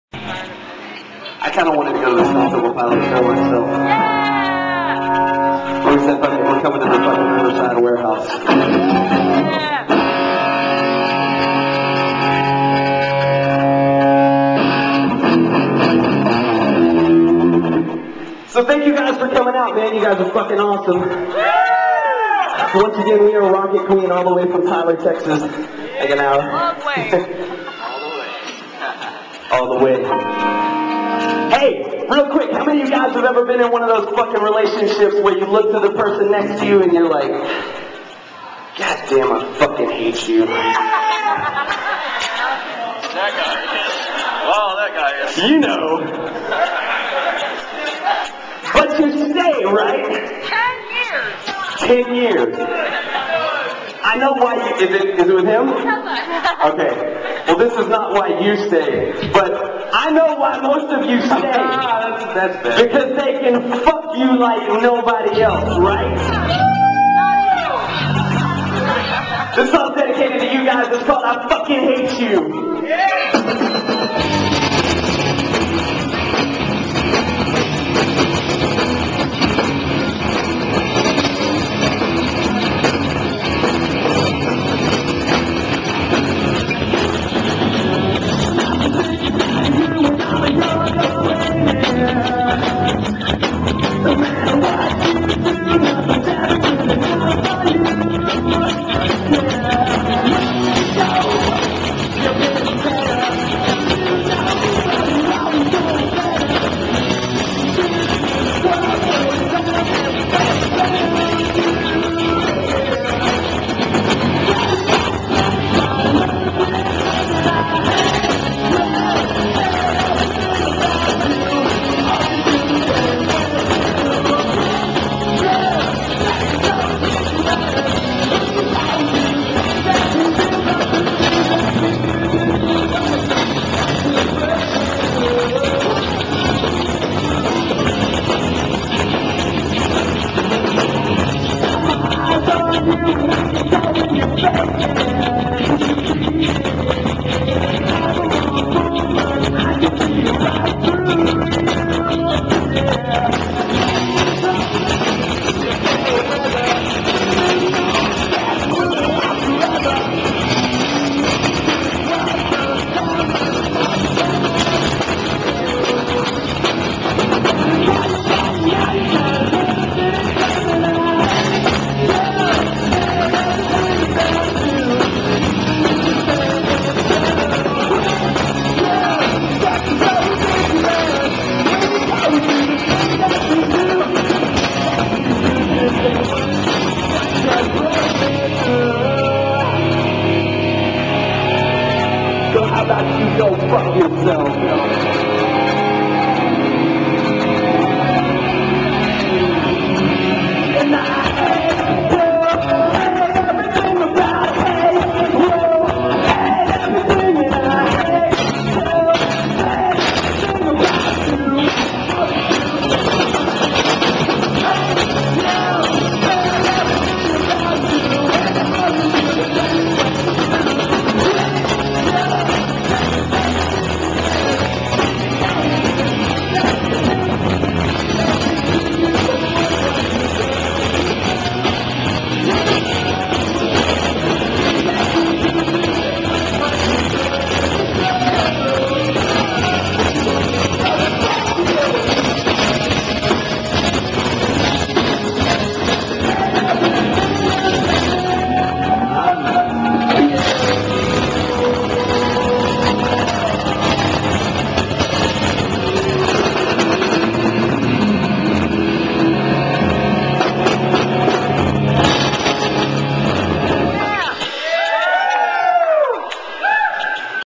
Rockett Queen - 99x STP after party (trimmed)
Recorded at the Riverside Warehouse on March 19, 2010 using a Flip Video camcorder.